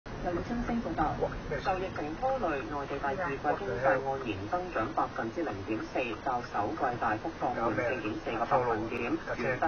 車CAM直擊 - 點解呢個迴旋處咁多人逆線行駛？
轉自: Keith